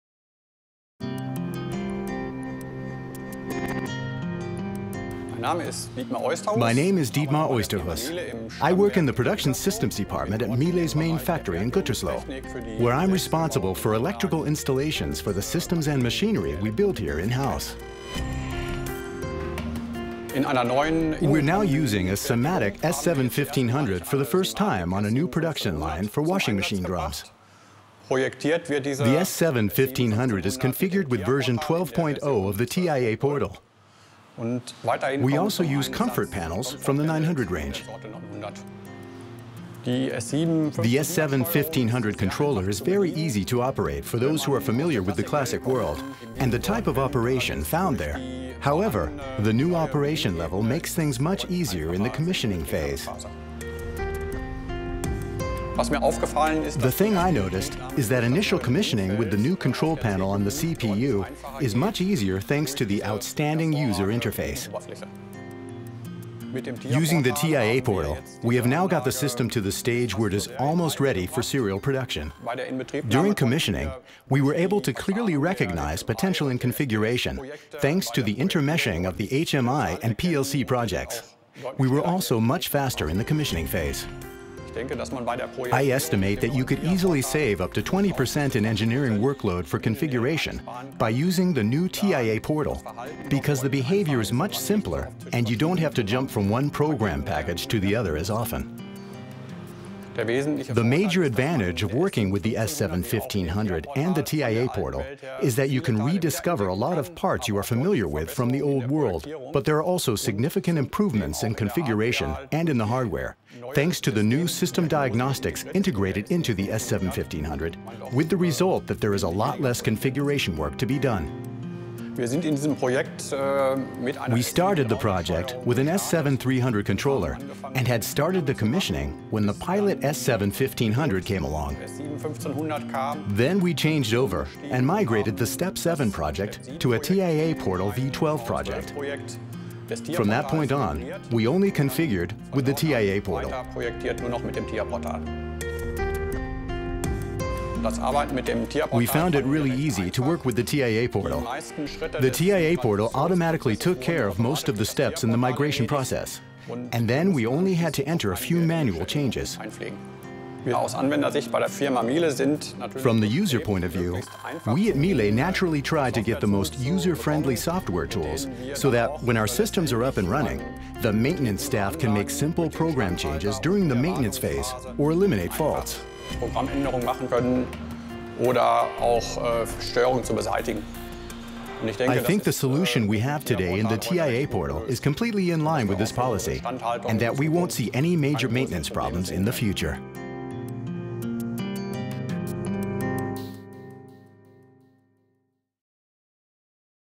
SIMATIC S7-1500 plus TIA Portal saves up to 20% on engineering
Be successful with innovative solutions - Miele, one of the leading manufacturers of household appliances, reports on the simple and fast commissioning of its new inner drum line production. Thanks to the perfect interaction of HMI and the S7-1500 CPU due to the common engineering framework TIA Portal Miele saves up to 20% in the engineering. That is engineering efficiency.